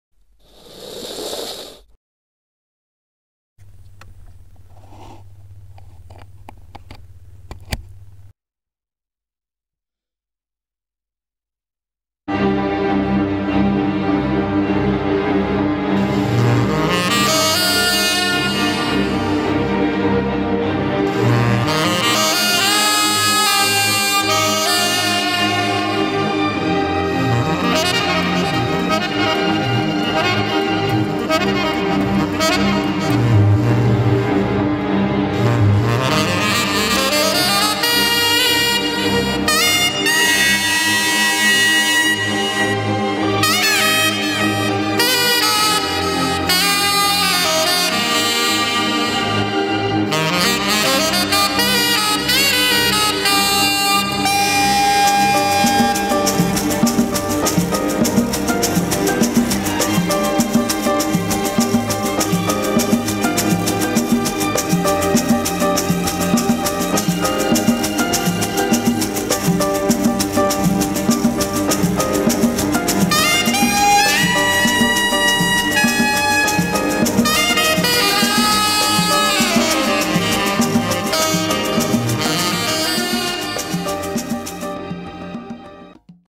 Saxophone recorded on iPhone 6- iRig PRE- AMT Quantum7 Wireless
Saxophone: VEGA
Microphone: AMT Quantum 7+ iRig PRE

GARAGE BAND